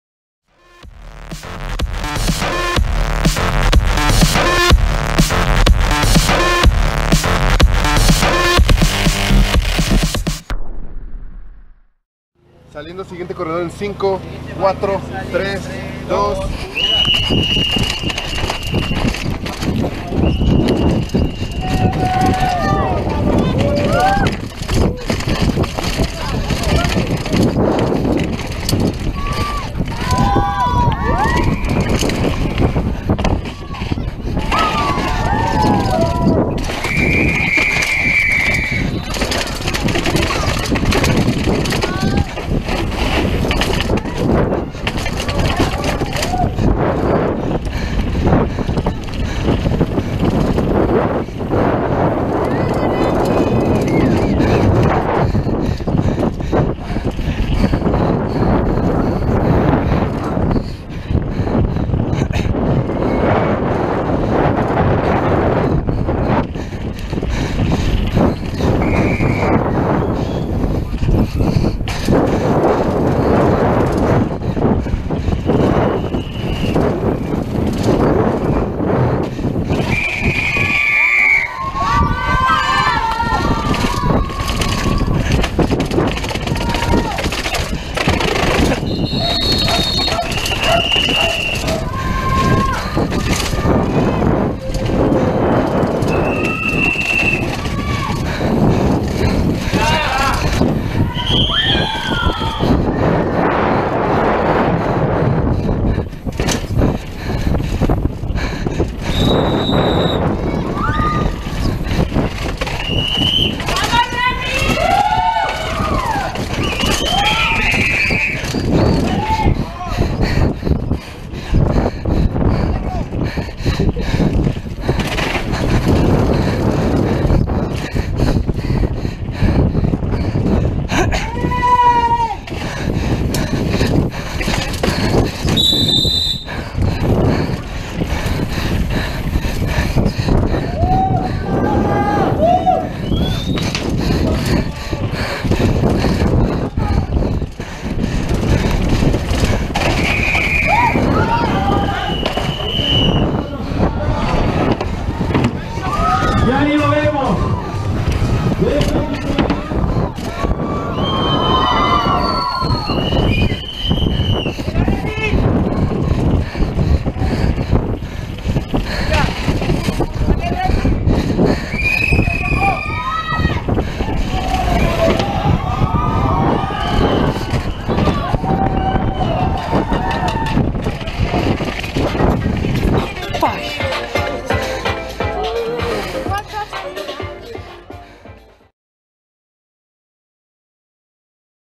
Race Run Downhill Taxco 2014 - Remy Metailler - Gopro POV
Here is my race run from Downhill Taxco 2014. 9th overall, 7s behind the win, less than 3 from the podium. A bit disappointed frommyself but still stoked on the event. Such an amazing feeling to ride down those streets !
@remymetailler on Instagram and Twitter.